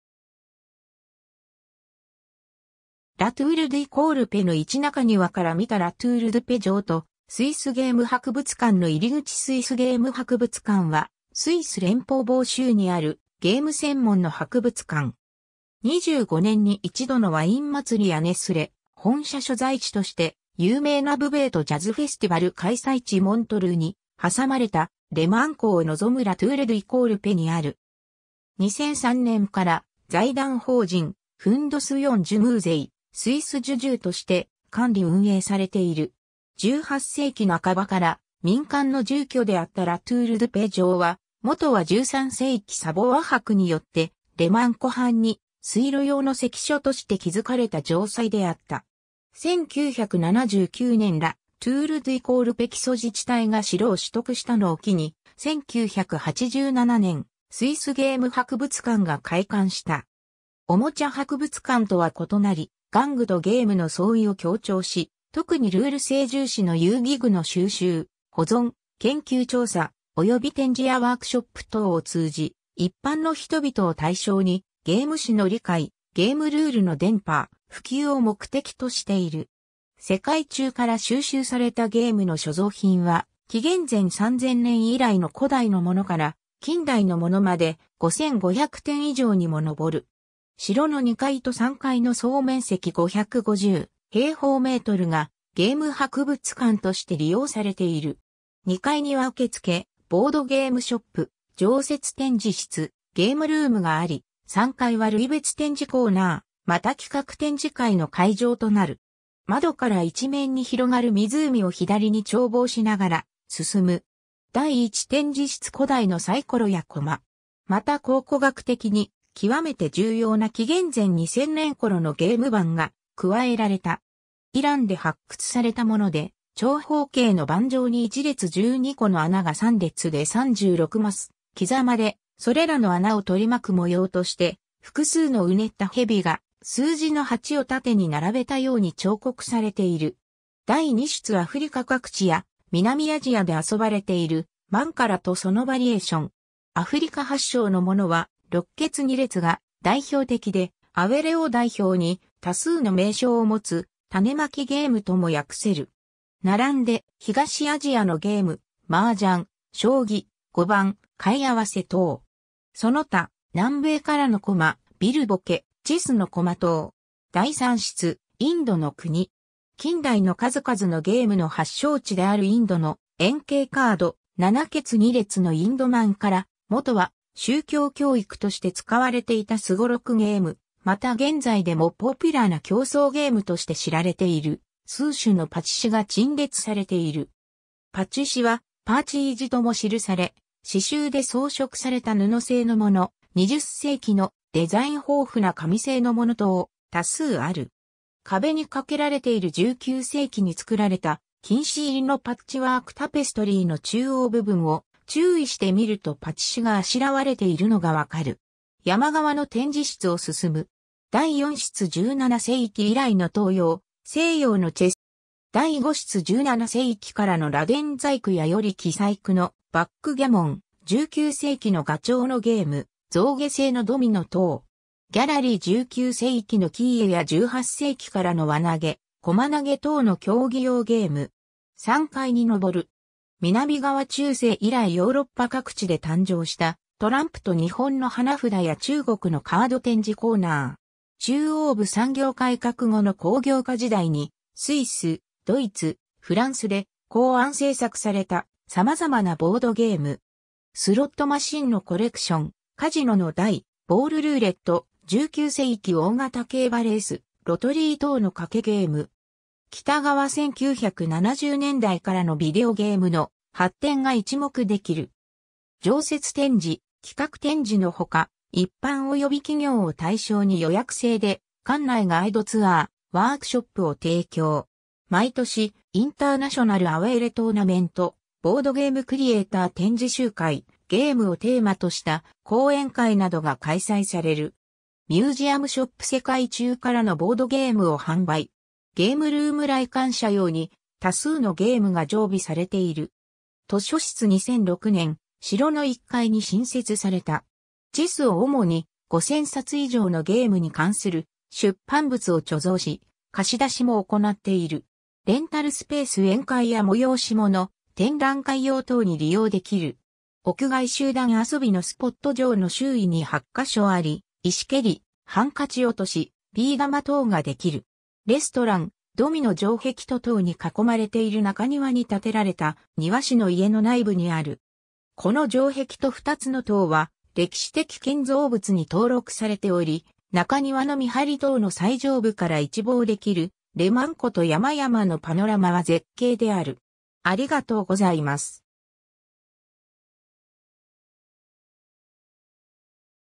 スイス・ゲーム博物館
ラ・トゥール＝ドゥ＝ペの位置 中庭から観たラ・トゥール＝ドゥ＝ペ城とスイス・ゲーム博物館の入口 スイス・ゲーム博物館（スイス・ゲームはくぶつかん、フランス語: Musée Suisse du jeu）は、スイス連邦ヴォー州にあるゲーム専門の博物館。
25年に一度のワイン祭りやネスレ本社所在地として有名なヴヴェイとジャズフェスティバル開催地モントルーに挟まれたレマン湖を臨むラ・トゥール＝ドゥ＝ペ (La Tour-de-Peilz) にある。
2003年から財団法人「 Fondation du Musée Suisse du jeu」として管理運営されている。
 18世紀半ばから民間の住居であったラ・トゥール＝ドゥ＝ペ城は、元は13世紀サヴォワ伯によってレマン湖畔に水路用の関所として築かれた城塞であった。
1979年ラ・トゥール＝ドゥ＝ペ基礎自治体（コミューン）が城を取得したのを機に、1987年スイス・ゲーム博物館が開館した。
 おもちゃ博物館とは異なり、玩具とゲームの相違を強調し、特にルール性重視の遊戯具の収集、保存、研究調査、及び展示やワークショップ等を通じ、一般の人々を対象にゲーム史の理解、ゲームルールの伝播、普及を目的としている。
 世界中から蒐集されたゲームの所蔵品は、紀元前3000年以来の古代のものから近代のものまで、5500点以上にも上る。
 城の2階と3階の総面積550平方メートルがゲーム博物館として利用されている。
2階には受付、ボードゲームショップ、常設展示室、ゲームルームがあり、3階は類別展示コーナー、また企画展示会の会場となる。
 窓から一面に広がる湖を左に眺望しながら進む。
 第一展示室 古代のサイコロや駒。
また考古学的に極めて重要な紀元前2000年頃のゲーム盤が加えられた。
イランで発掘されたもので、長方形の盤上に1列12個の穴が3列で36枡刻まれ、それらの穴を取り巻く模様として、複数のうねった蛇が数字の8を縦に並べたように彫刻されている。
 第二室 アフリカ各地や南アジアで遊ばれている「マンカラ」とそのバリエーション。
アフリカ発祥のものは、6穴2列が代表的で、アウェレを代表に多数の名称を持つ、種まきゲームとも訳せる。
並んで東アジアのゲーム、麻雀、将棋、碁盤、貝合わせ等。
その他、南米からの独楽、ビルボケ（けん玉）、チェスの駒等。
 第三室 「インドの国」。
近代の数々のゲームの発祥地であるインドの円形カード、7穴2列のインドマンカラ、元は宗教教育として使われていたすごろくゲーム、また現在でもポピュラーな競争ゲームとして知られている数種のパチシが陳列されている。
パチシはパーチージとも記され、刺繍で装飾された布製のもの、20世紀のデザイン豊富な紙製のもの等、多数ある。
壁に掛けられている19世紀に作られた金糸入りのパッチワークタペストリーの中央部分を注意して見るとパチシがあしらわれているのが判る。
 山側の展示室を進む。
 第四室 17世紀以来の東洋、西洋のチェスボードと多種多様に形作られた駒。
 第五室 17世紀からの螺鈿細工や寄木細工のバックギャモン、19世紀の鵞鳥のゲーム、象牙製のドミノ等。
 ギャラリー 19世紀のキーユ（木製ボウリング）や18世紀からの輪投げ、駒投げ等の競技用ゲーム。
 3階に上る。
 南側 中世以来ヨーロッパ各地で誕生したトランプと日本の花札や中国のカード展示コーナー。
 中央部 産業改革後の工業化時代にスイス、ドイツ、フランスで考案製作された様々なボードゲーム。
スロットマシンのコレクション、カジノの台『ボールルーレット』、19世紀大型競馬レース、ロトリー等の賭けゲーム。
 北側 1970年代からのビデオゲームの発展が一目できる。
 常設展示、企画展示の他、一般及び企業を対象に予約制で館内ガイドツアー、ワークショップを提供（有料）。
 毎年、インターナショナル・アウェレトーナメント（マンカラ）、ボードゲームクリエーター展示集会、ゲームをテーマとした講演会などが開催される。
 ミュージアムショップ 世界中からのボードゲームを販売。
（出入り自由） ゲームルーム 来館者用に多数のゲームが常備されている。
（非電源ゲーム） 図書室 2006年、城の1階に新設された。
チェスを主に、5000冊以上のゲームに関する出版物を貯蔵し、貸し出しも行っている（要予約）。
 レンタルスペース 宴会や催し物、展覧会用等に利用できる。
 屋外集団遊びのスポ...